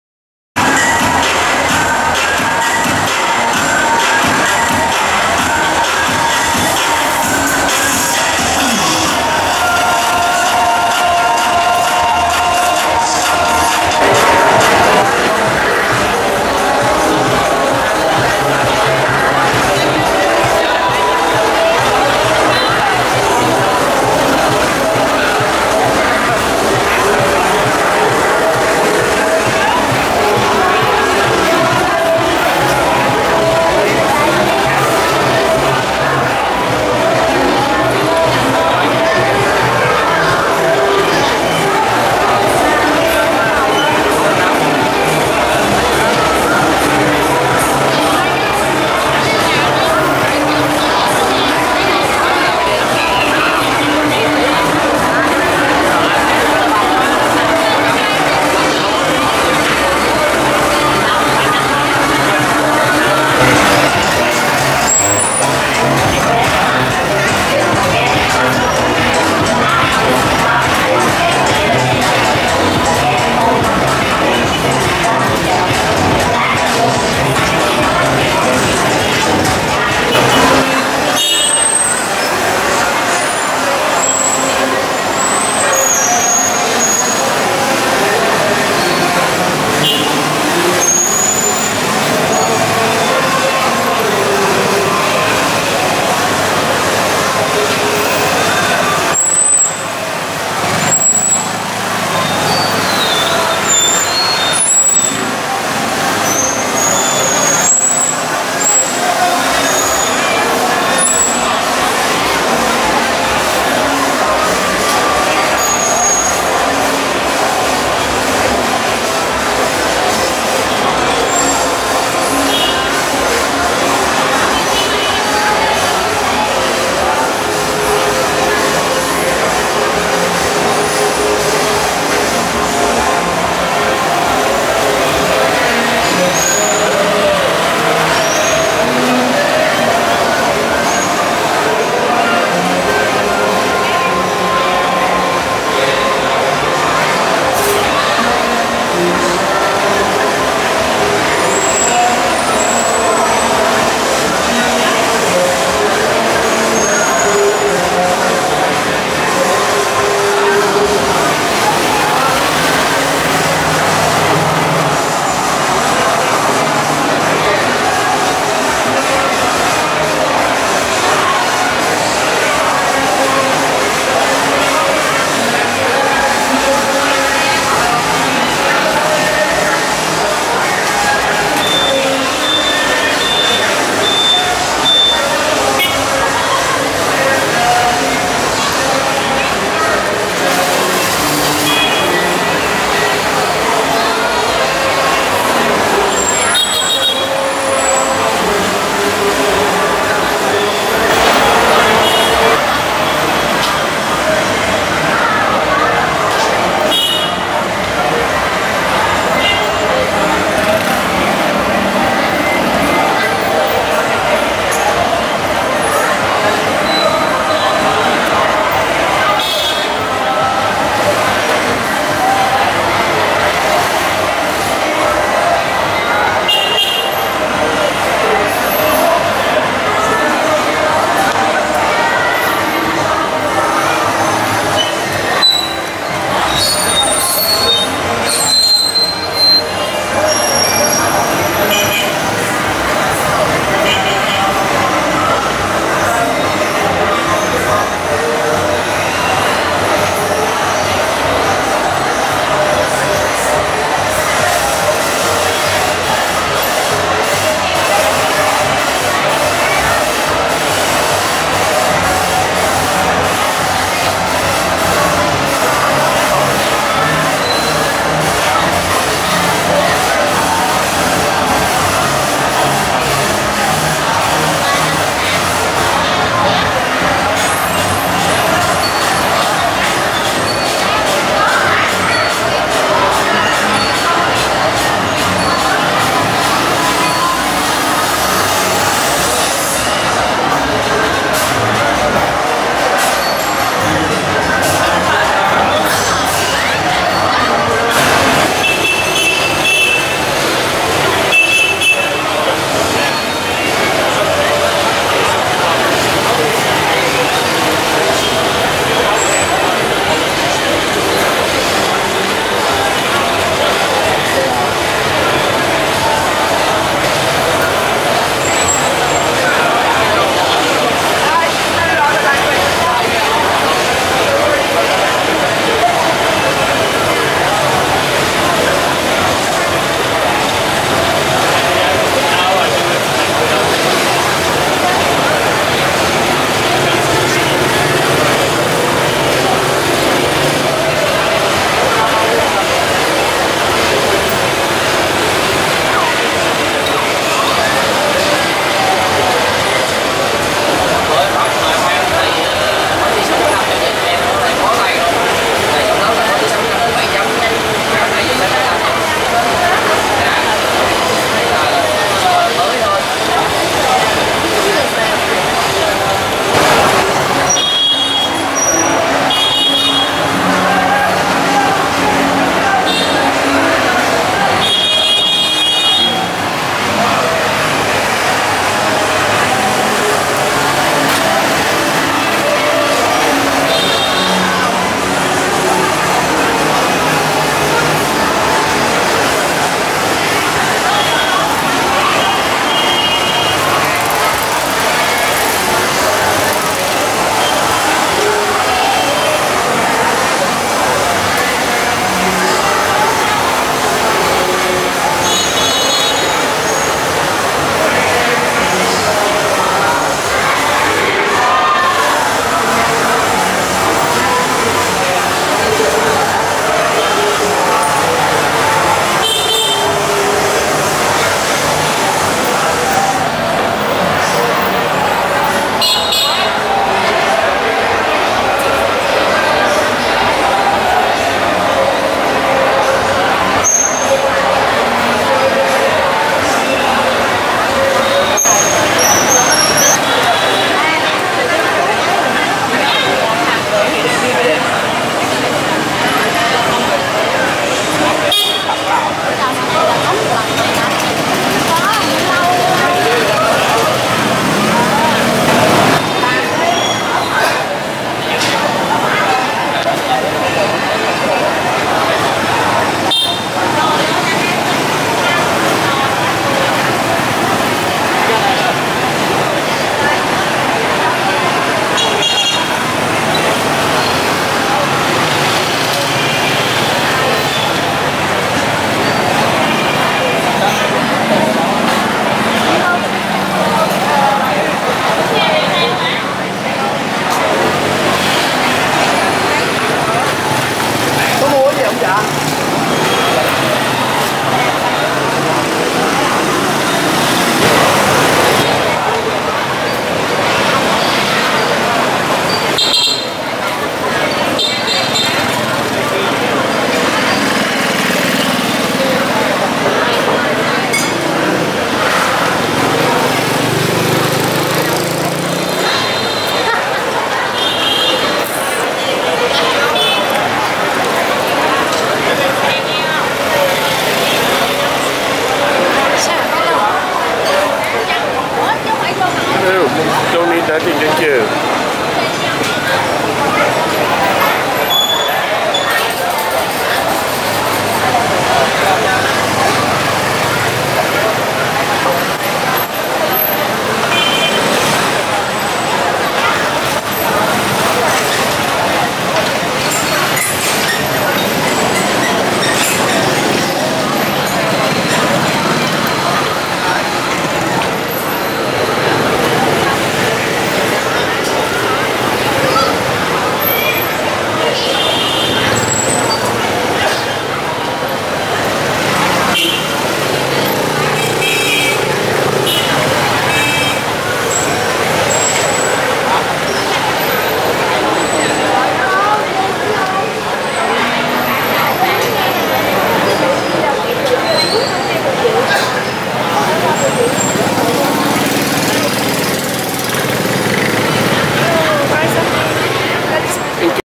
HO CHI MIN(SAIGON)NIGHTLIFE-BUI VIEN BARS HOSTESS WORKING GIRLS
Amazing scenes of Saigon most famous street